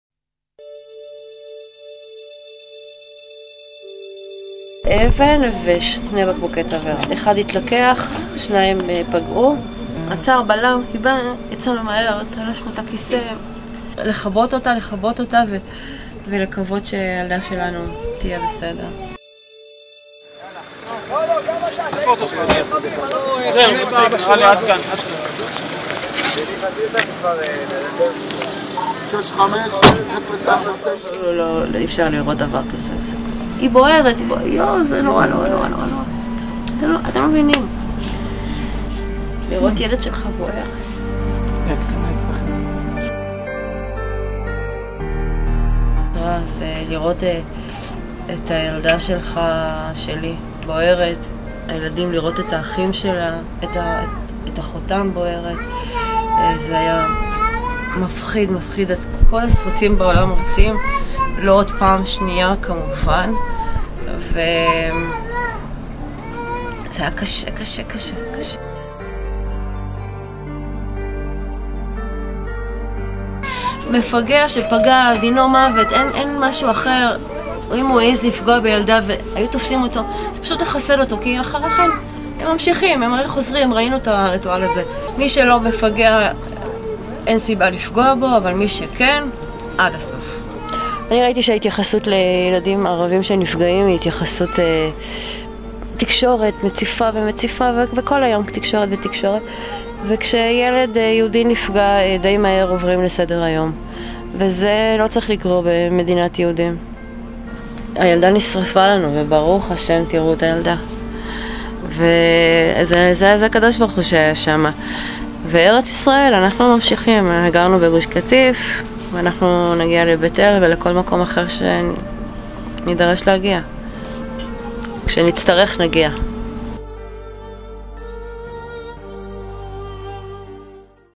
"הילדה נשרפה לנו וב"ה בחיים"
סיגל סופר בראיון וידאו מרגש על האירוע שהתרחש ביום שישי האחרון. "זה נורא לראות את הילדה שלי בוער.."